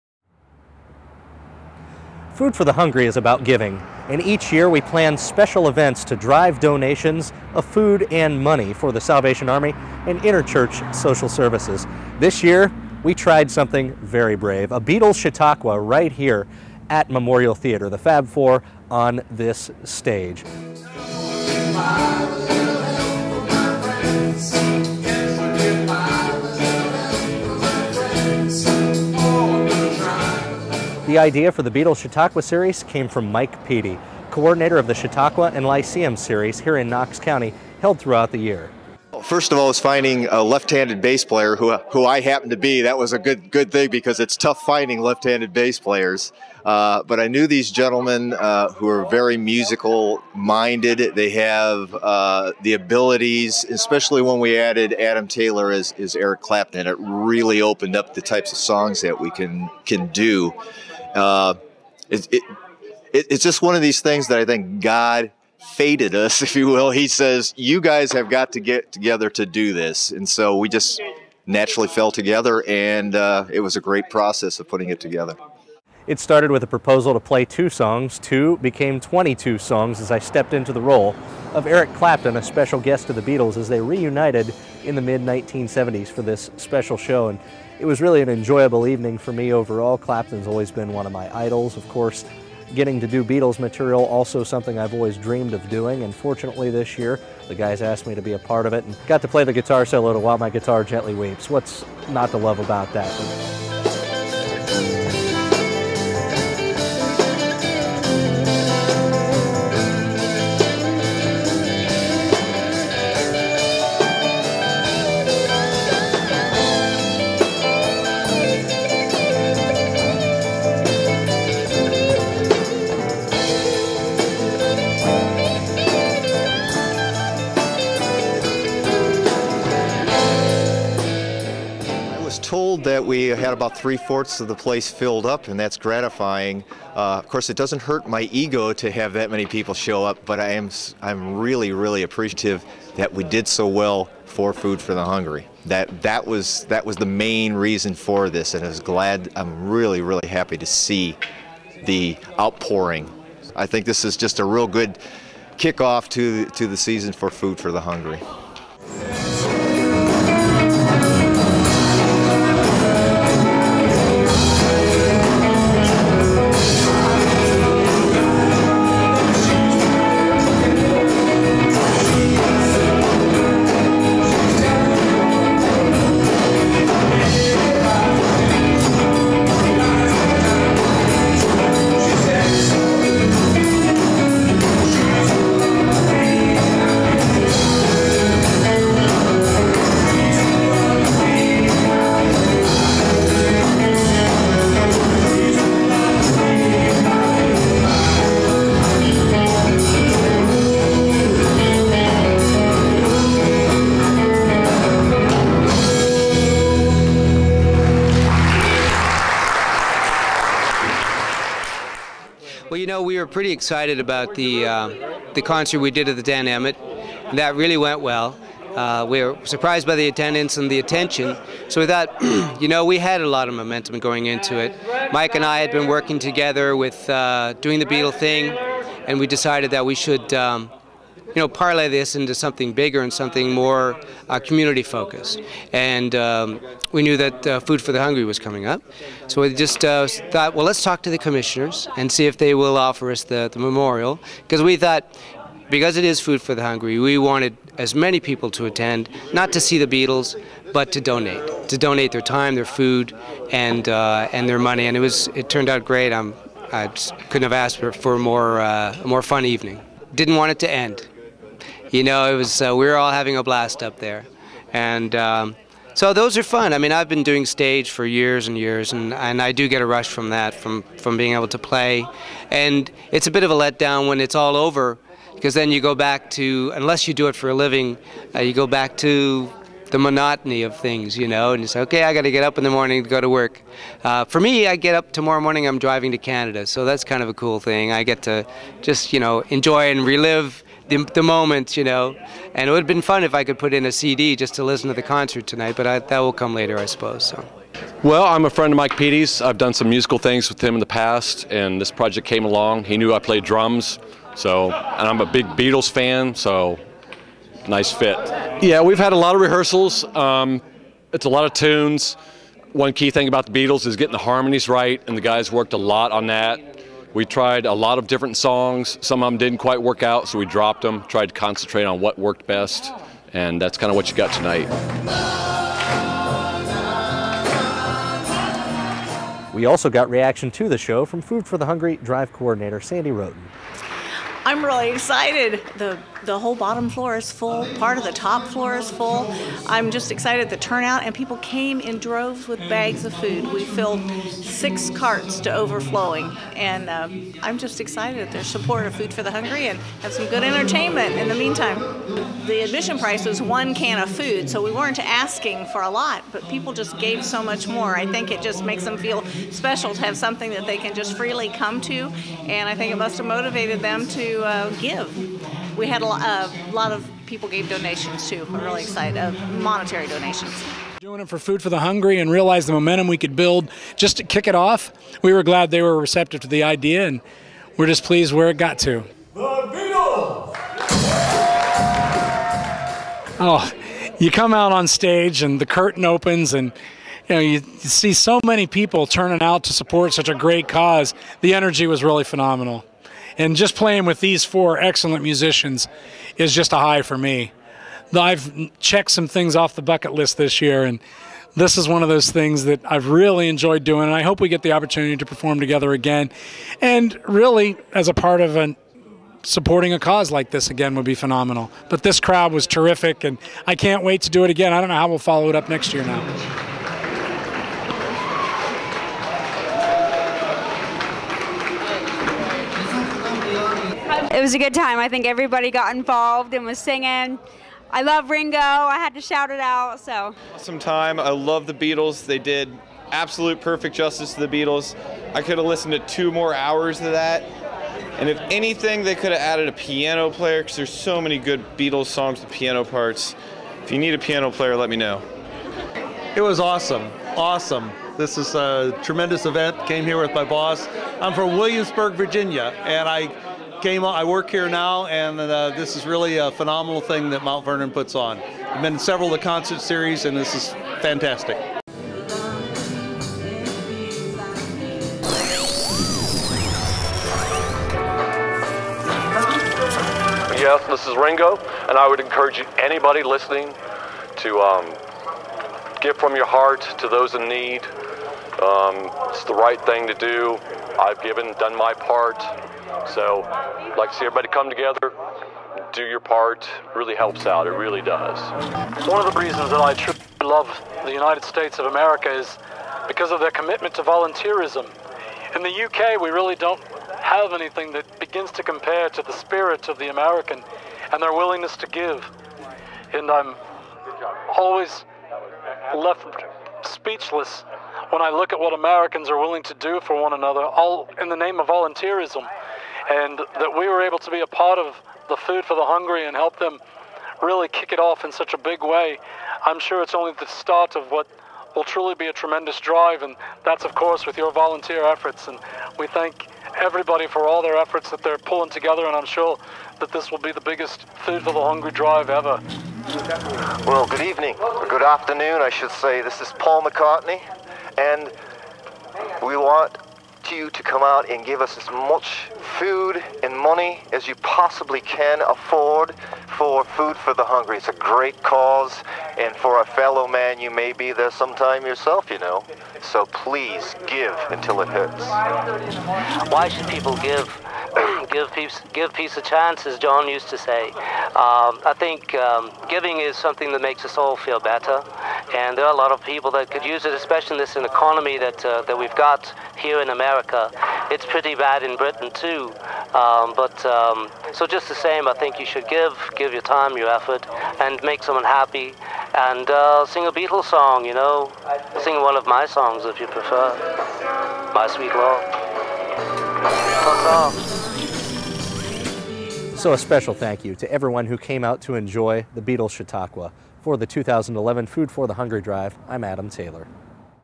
Mt. Vernon Beatles Chautauqua
Recap of the Mount Vernon Beatles Chautauqua to benefit Food For The Hungry.